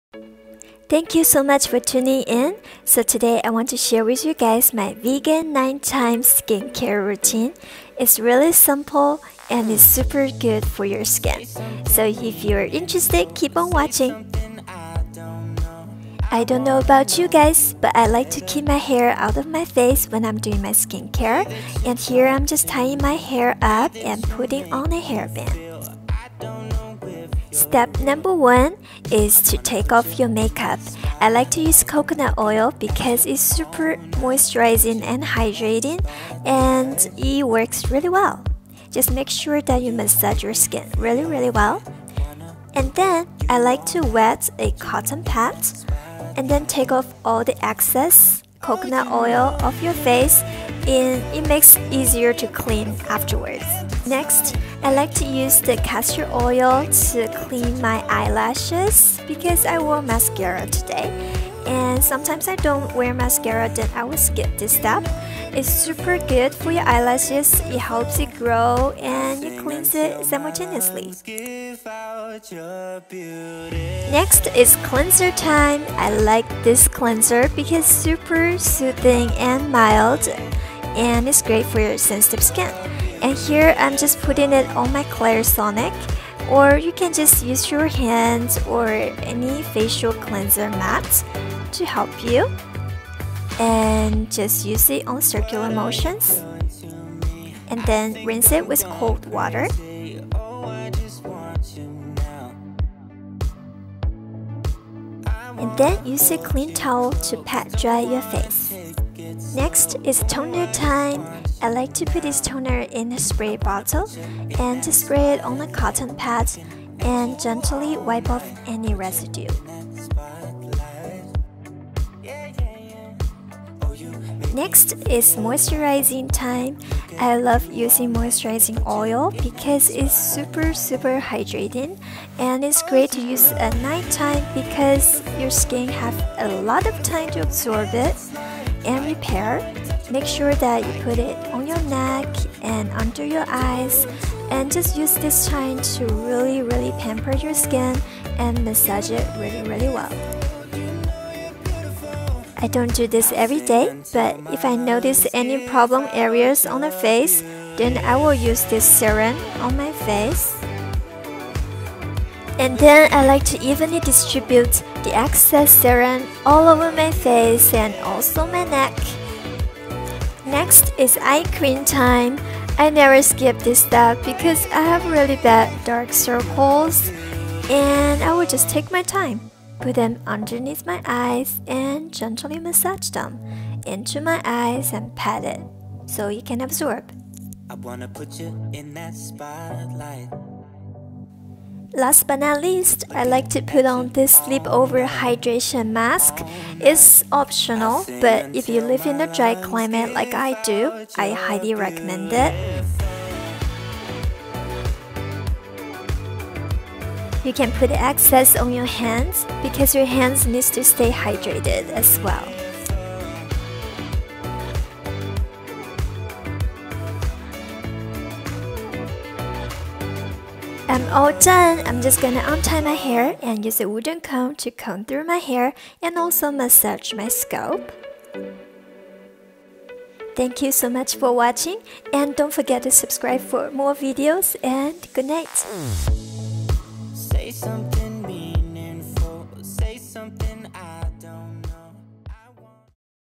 My Night Skincare Routine 2018 (Vegan, All Natural, Non-Toxic)
In this updated Night Skincare Routine, I will show you a few simple steps you can do every night to achieve a youthful and radiant skin. All of the products used are vegan, cruelty free and non-toxic. I hope you find this video helpful, let me know if you have any requests, question or comments. I enjoy connecting with you and getting to know you MORE.

Love & Light,
Lan (aka CeCe)

(NEW VID every Monday, Wednesday and Saturday at 3:00 pm PST)
🔶Here are the Products (or similar products) used in My Routine🔶

1.Organic Extra Virgin Coconut Oil

2.Organic Castor Oil

3. Organic Cotton Pads

4.Pacifica Quinoa Sensitive Super Gentle Face Wash

5. Clarisonic Mia Facial Cleansing Brush

6. Thayers Rose Petal Witch Hazel

7. Living Libations Seabuckthorn Best Skin Ever

8. Living Libations Rose Glow Serum

9.100% Pure Coffee Bean Caffeine Eye Cream

10. Pacifica Wakeup Beautiful Sleepover Mask

✨Soul Pure Mind (Tips to UPGRADE Your Life & Relationships)